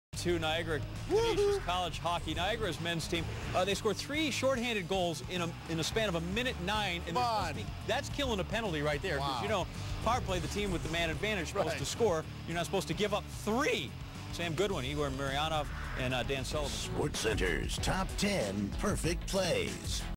Niagara Hockey Scores Three Shorthanded Goals On One Penalty
Niagara scored three-shorthanded goals on the same two-minute minor against rival Canisius, a game that Niagara won 5-2 on Nov. 11, 2008.